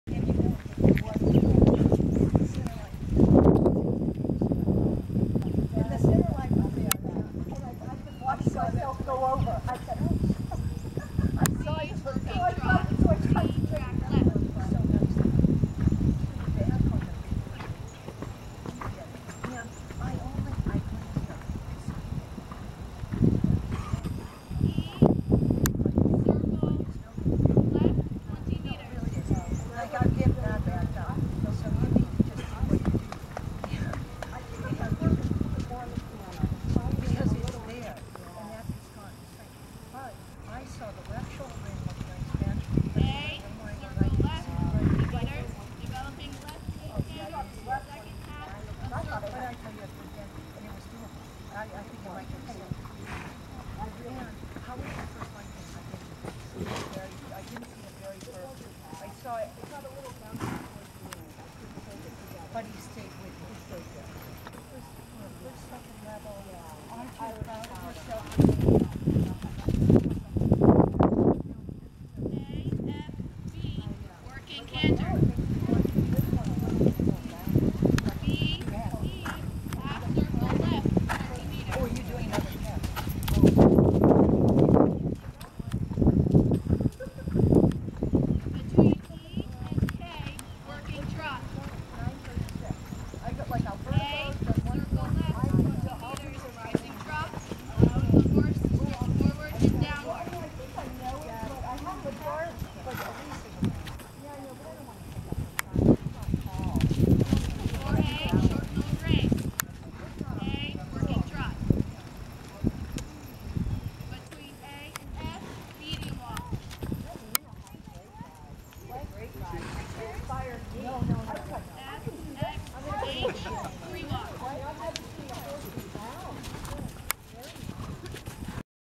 Tequila Dressage Training Level Test One
The camera ran out of batteries, but Casey had more in her pocket and changed them quickly!  The next video show the completion of the test.